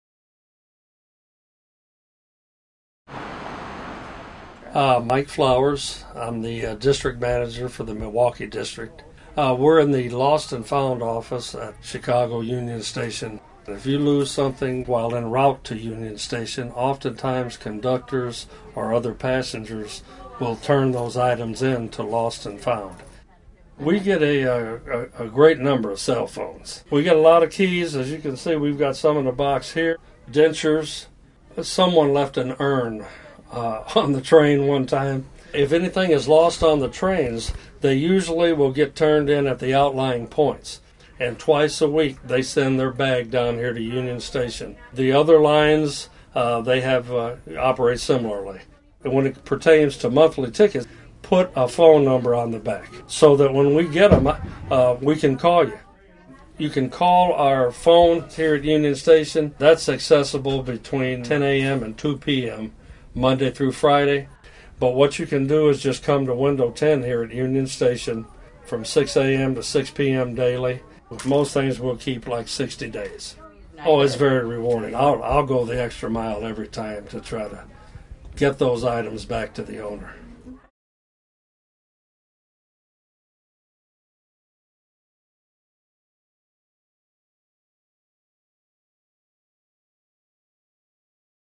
A Behind the Scenes Look: Lost & Found
Items lost on the train, platform or other railroad property land in the Lost and Found offices at four of Metra’s downtown Chicago stations. Look for items you might have lost in the office in the station where your line terminates.